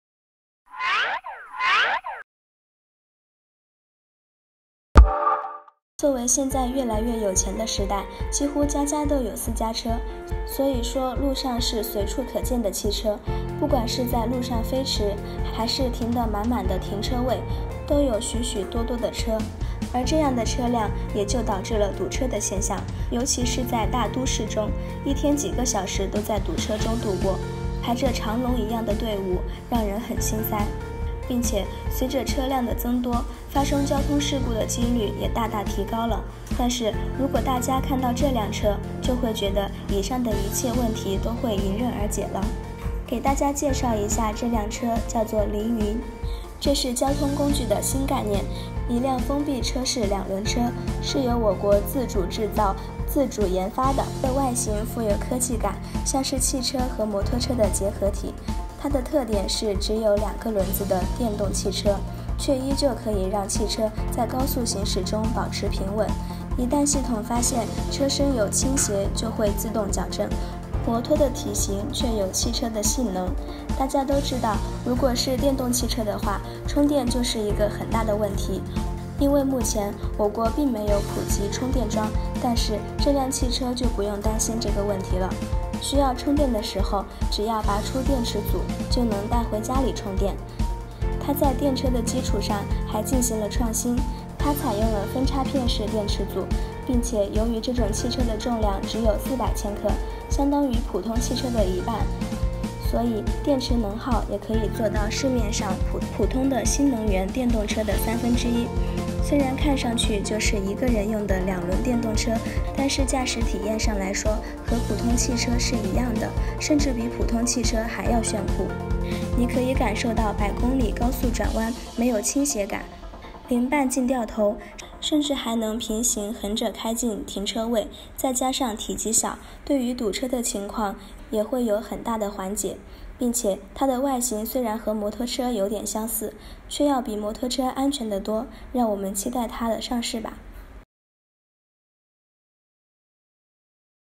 交通工具的新概念，封閉車身式兩輪車，摩托的體型汽車的性能
作為現在越來越有錢的時代幾乎家家都有私家車了，所以說路上是隨處可見的汽車，不管是在路上飛馳的還是停的滿滿的停車位，都有許許多多的車，而這樣的車輛也就導致了堵車的現象。尤其是在大都市中，一天幾個小時都是在堵車中度過，排著長龍一樣的隊伍，很是心塞。並且隨著車輛的增多，發生交通事故的幾率也被大大的提高。但是如果大家看到這輛車，就會覺得以上的一切問題都迎刃而解了。